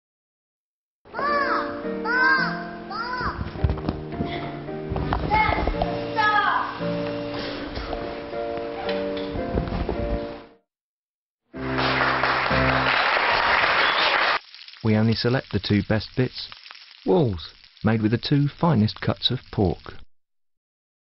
Walls. Two best bits. Nativity.
Nativity play ad from Walls Sausages "We only select the two best bits" campaign UK. By Beattie McGuinness Bungay